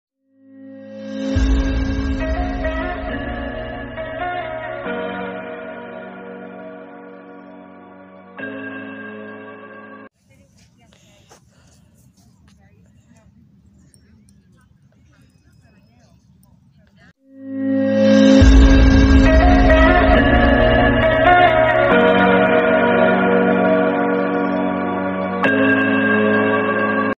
Mello meets another beagle (rare)
this is rare for her lol both are females lol